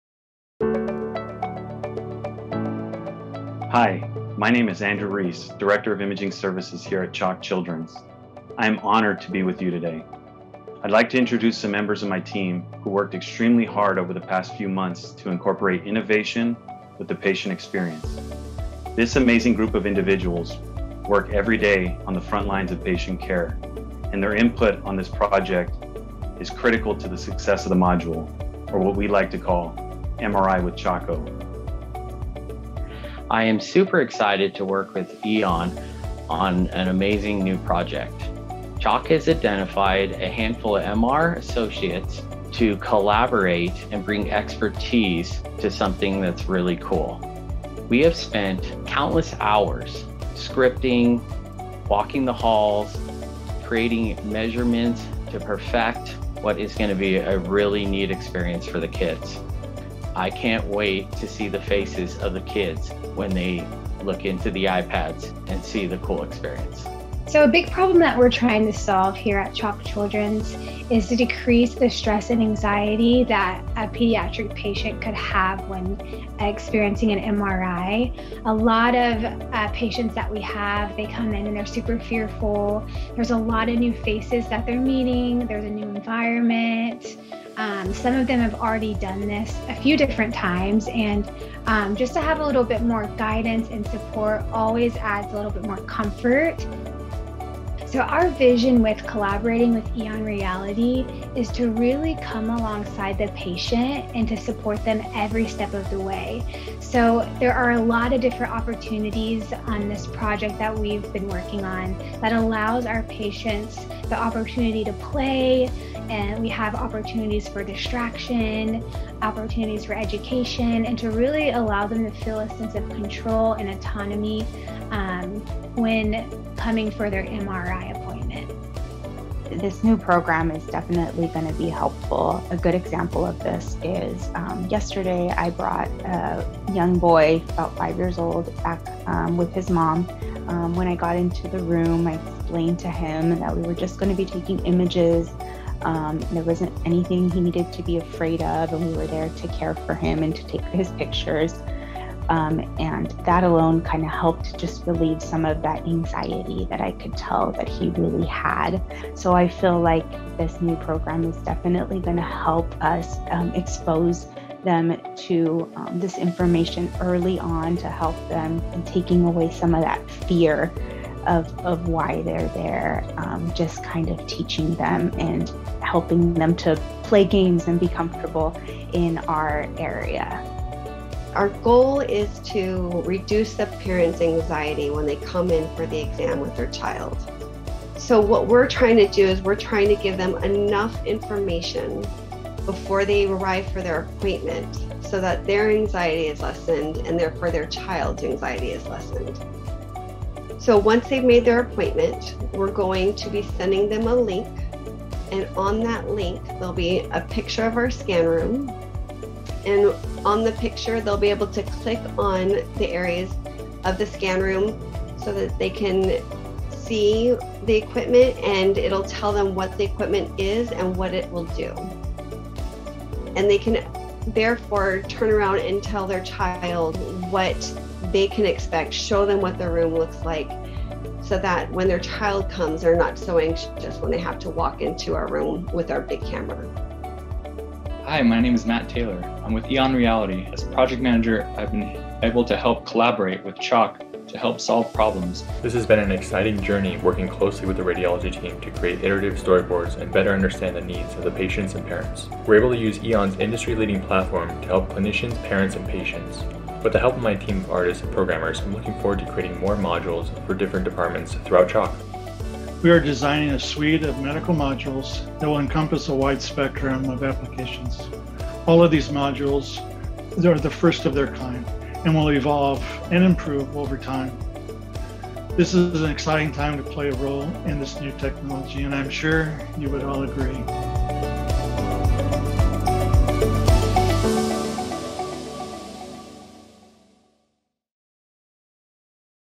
CHOC - MRI with Choco
An augmented reality module to provide comfort for patients and parents in the form of an informative and entertaining experience. The goal  is to decrease the stress and anxiety that a pediatric patient and patents could have when experiencing an MRI. By giving them enough information  before they arrive for their appointment, their anxiety is lessened and therefore their child's anxiety is lessened.